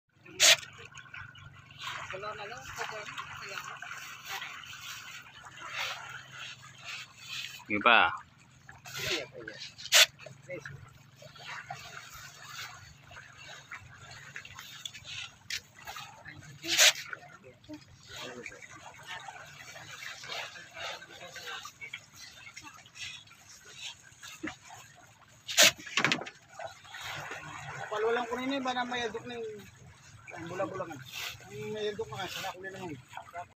Fish Farming | Feeds Starter Tilapia.
Practice na si pachok mag feeding fish